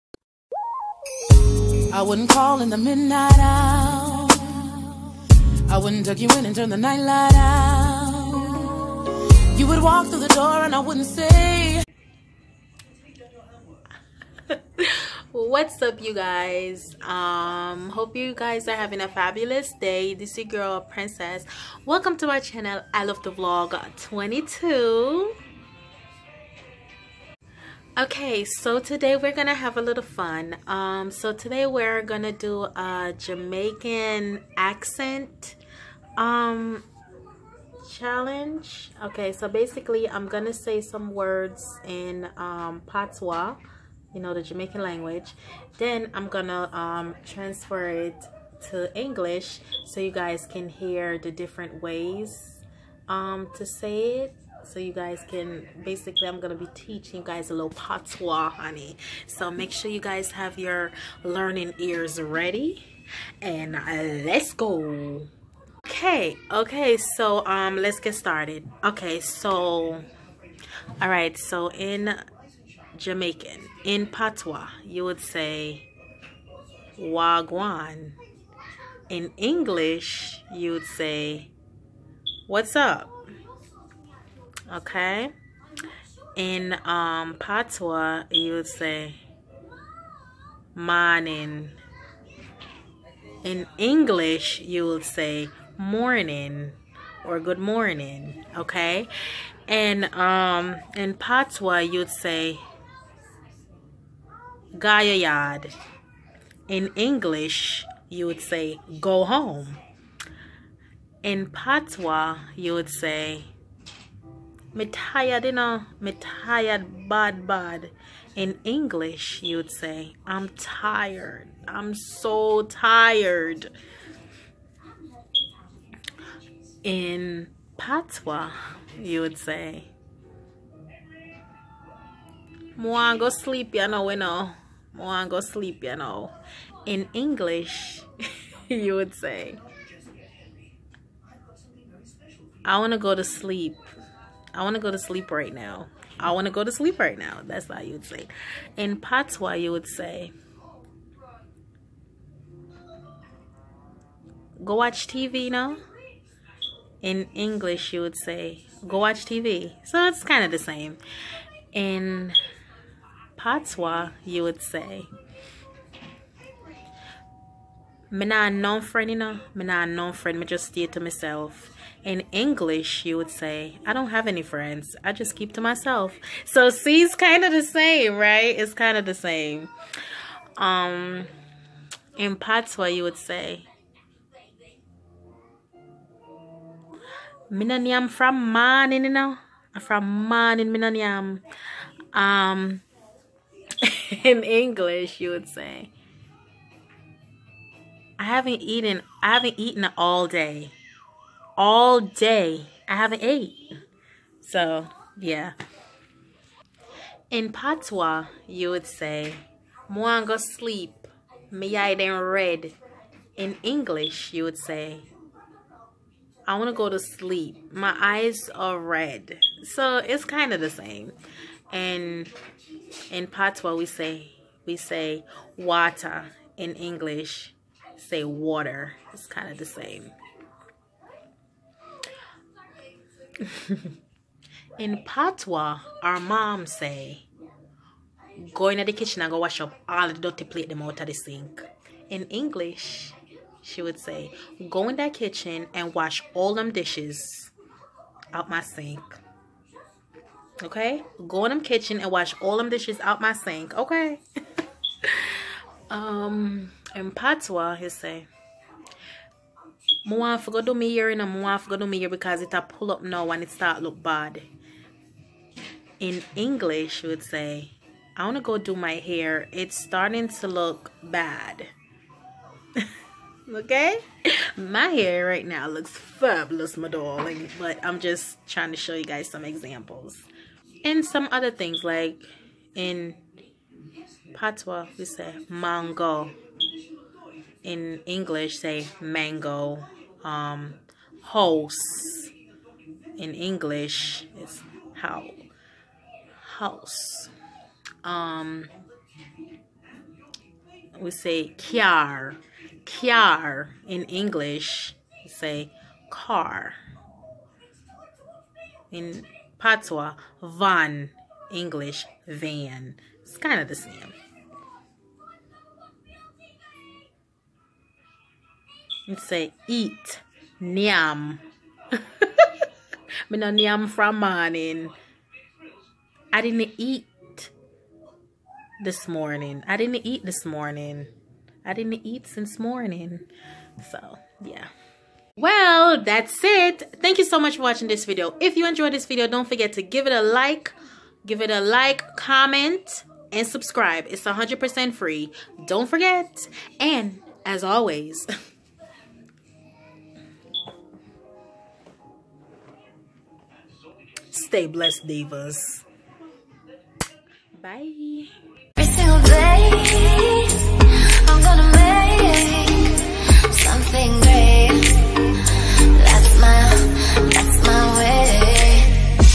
ACCENT CHALLENGE THIS IS LIT CHECK IT OUT| ilovetovlog22
Hey everyone back with another video
Today we are doing something new
I will be translating Jamaican patwah to American English.

Note: i am Jamaican
I left Jamaica at age 11
My language is a little different because all Jamaicans do not talk the same.